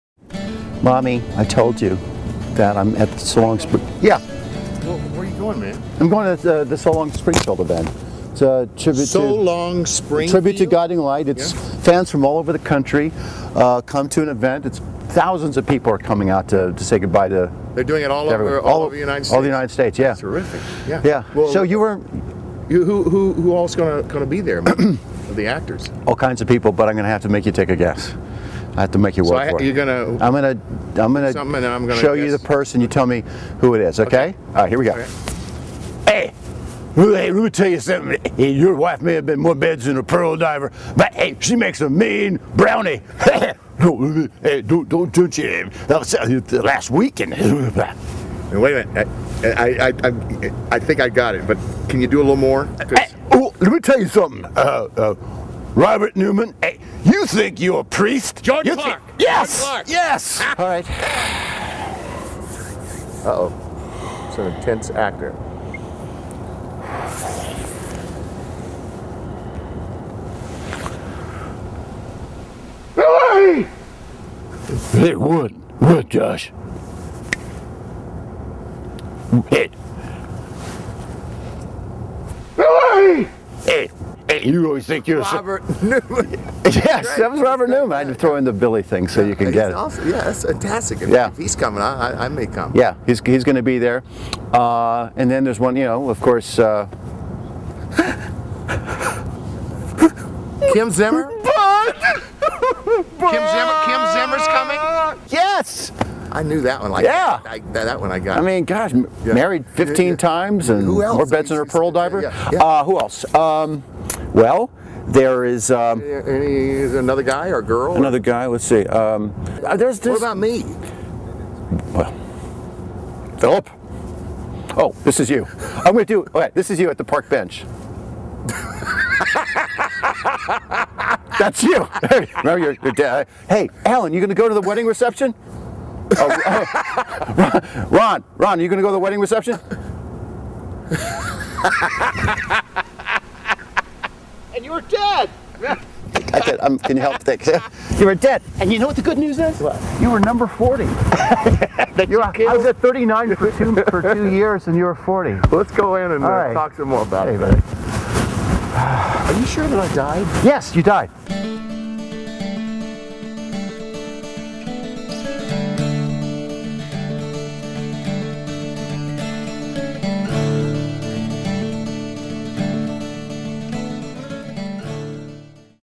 So Long Springfield
Michael O'Leary talks with Ron Raines about So Long Springfield and does impersonations of fellow cast members.